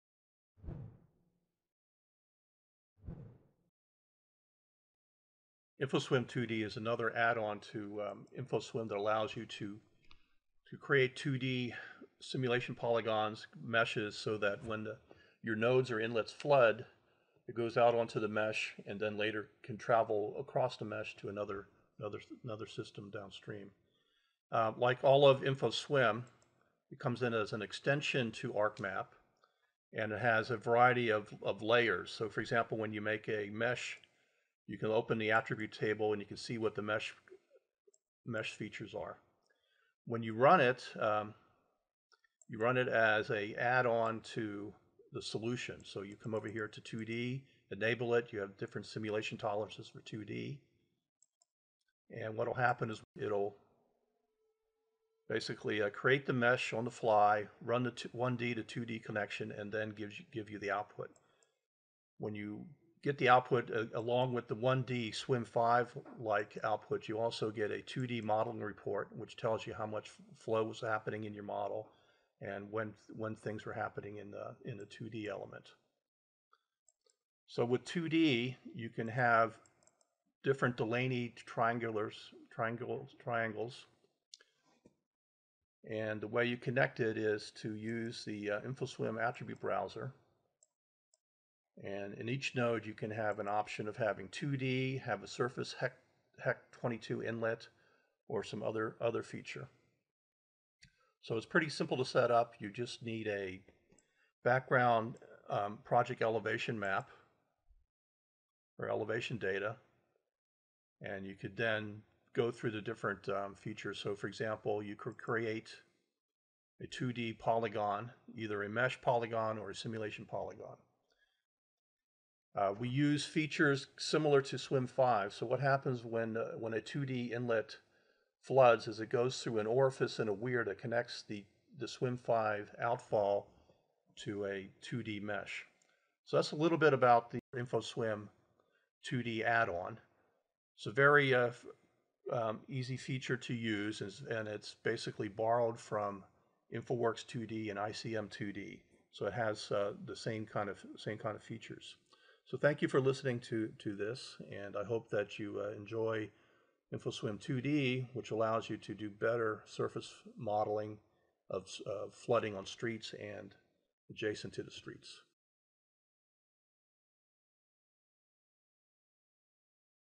Introduction to InfoSWMM 2D (Surface Flow Modeling)
InfoSWMM 2D lets you simulate, analyze and predict potential flood extents, depth and velocity, and accurately model the interaction of surface and underground systems in a fully integrated 1D/2D environment.